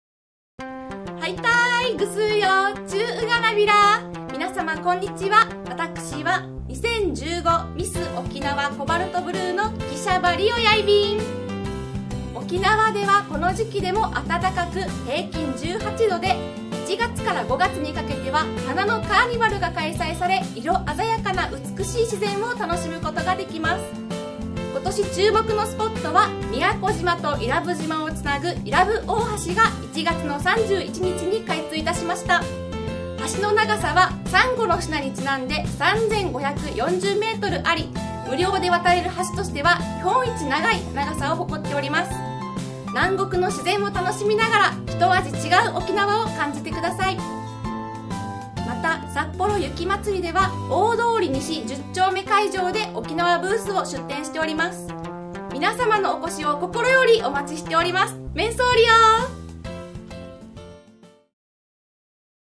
ミス沖縄コバルトブルーの喜舎場梨予さんが沖縄観光をPR
2015ミス沖縄コバルトブルーの喜舎場梨予さんが沖縄観光をPR